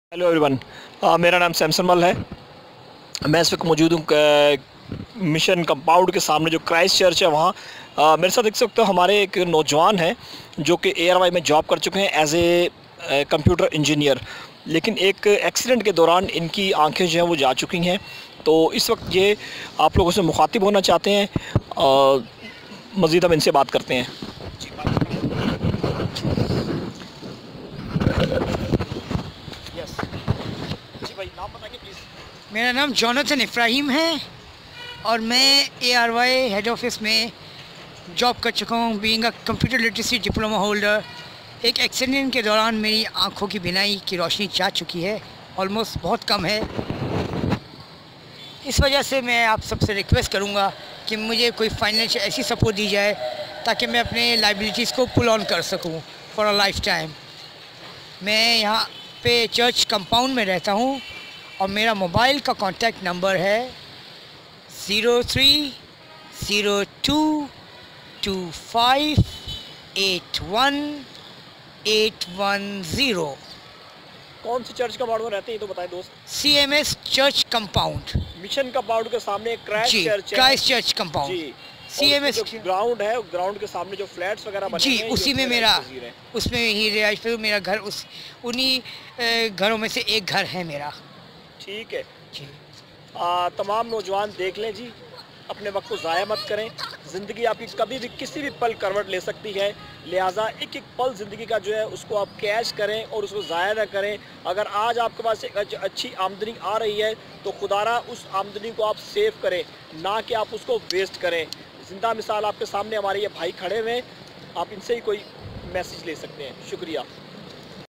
Jesus Life TV
Mr jonajhan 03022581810 اس نوان ایک روڈ اکسیڈنٹ میں رونوں آنکھوں سے مہروم  ہو گیا ہےآپ سب سے مرت کی درخواست یے